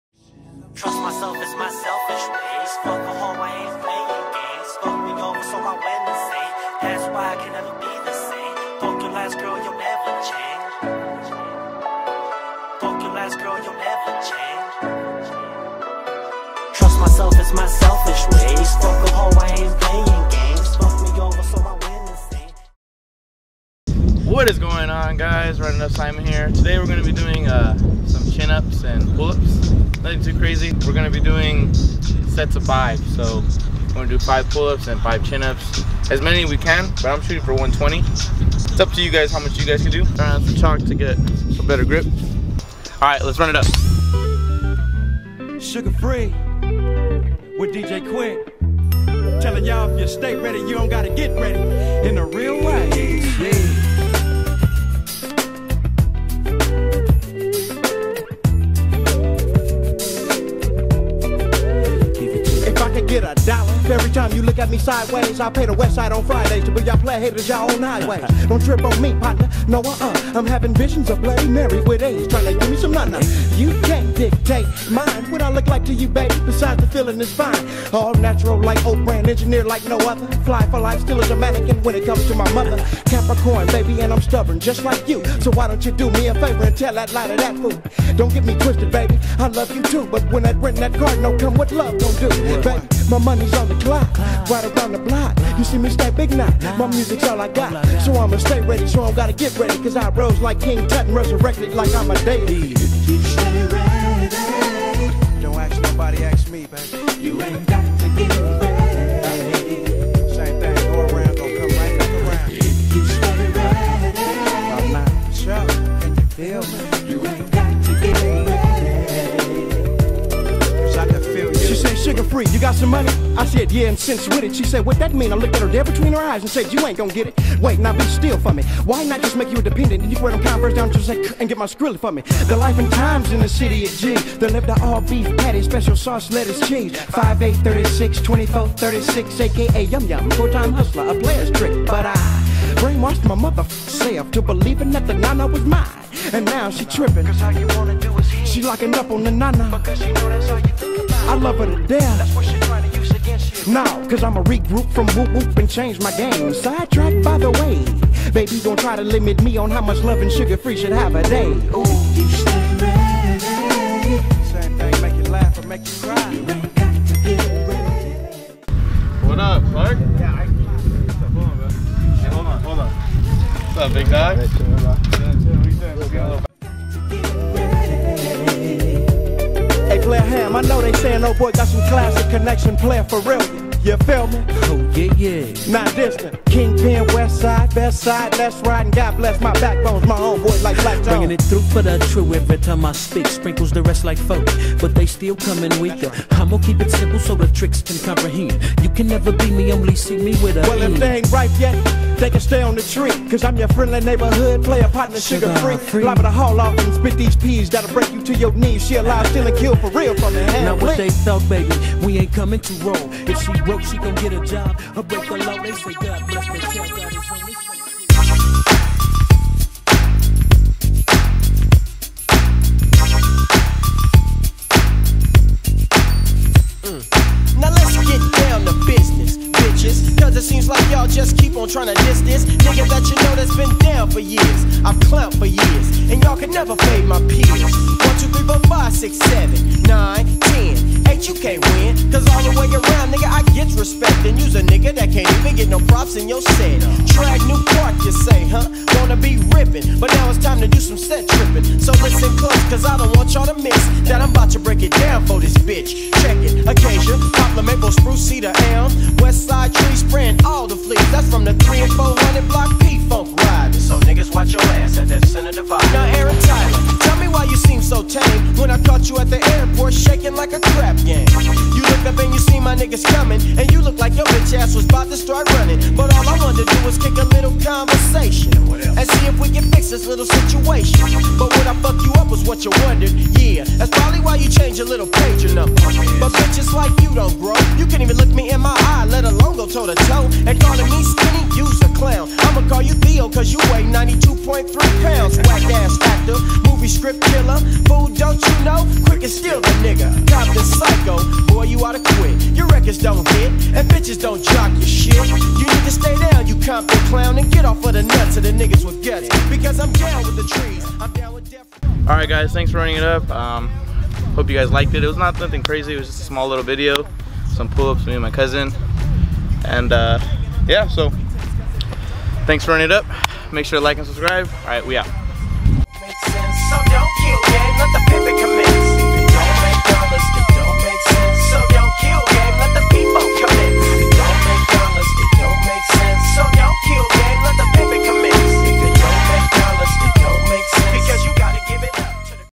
30 min back & bicep workout! (No equipment needed)
Do as many as you can. Im just showing how much I could do.
Let me know how many you did, and how you liked it.